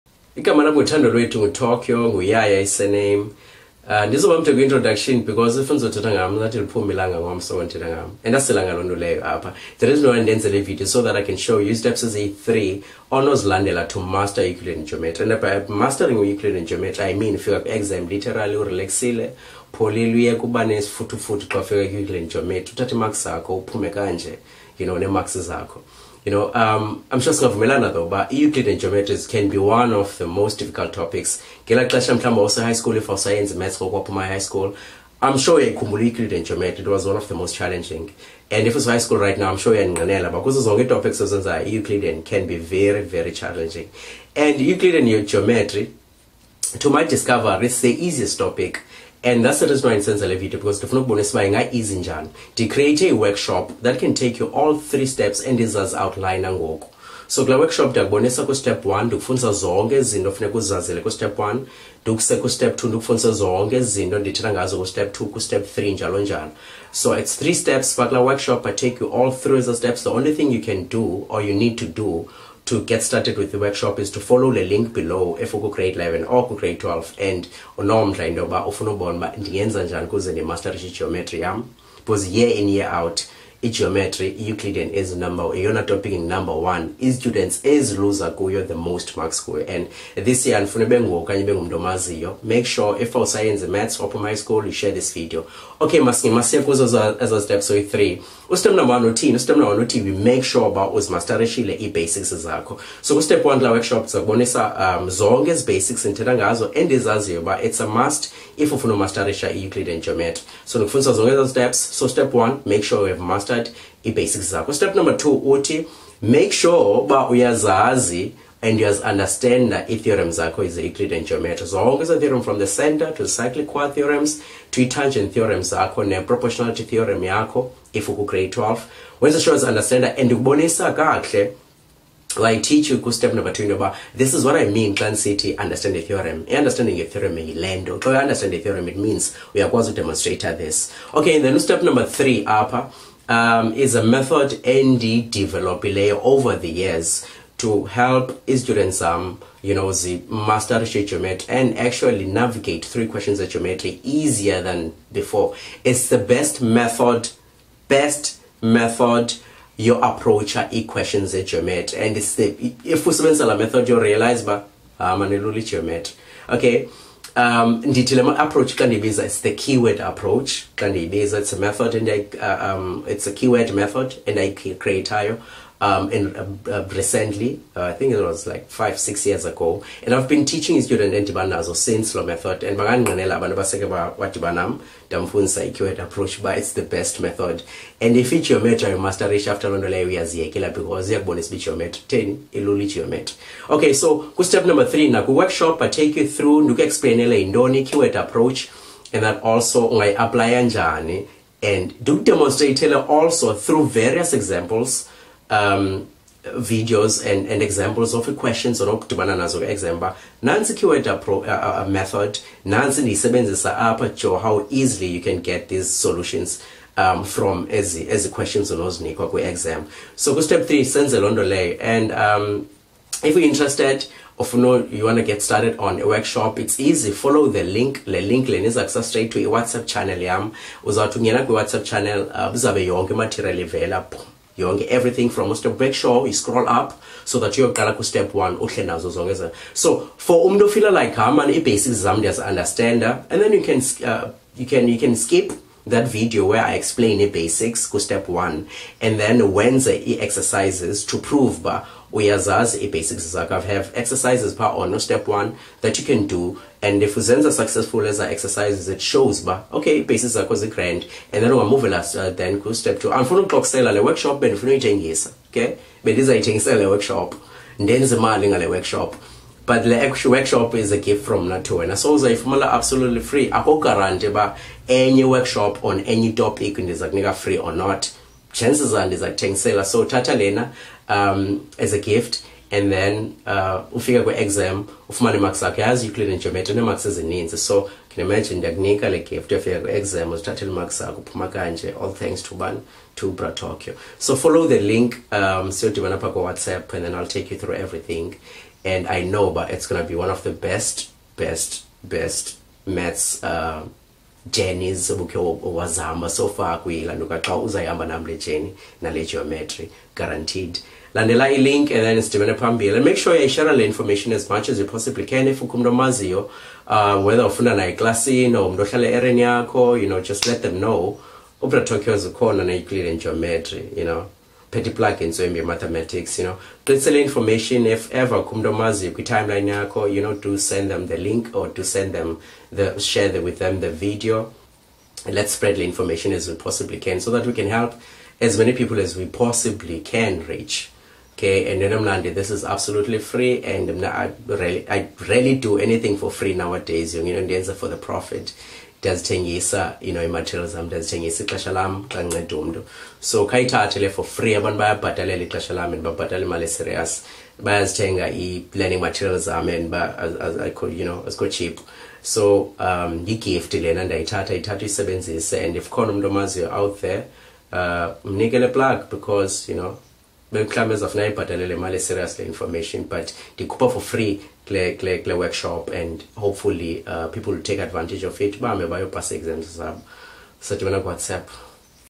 How 2 Master Euclidean Geometry in 3 Steps
🔌Euclidean Geometry Workshop (Grade 11 &12)

Maths students lose most marks in Euclidean Geometry.  It is regarded as the most difficult topic in maths.

I created a workshop to teach students a method I created to easily solve geometry problems.

My method is the Best!!

I have shared the method with my students over the past couple of years and it works. Learn the method and you're guaranteed to improve your geometry skills and score more marks in exams.

Follow the link below and get started right now!